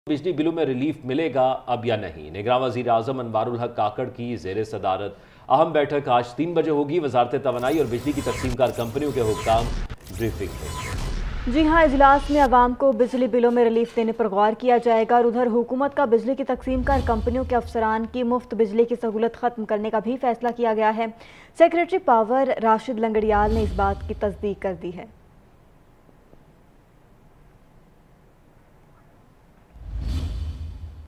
Big Decision Of Caretaker Prime Minister | Breaking News | PNN News
عوام کو بجلی بلوں میں
ریلیف ملے گا یا نہیں؟
اہم بیٹھک بڑے فیصلے !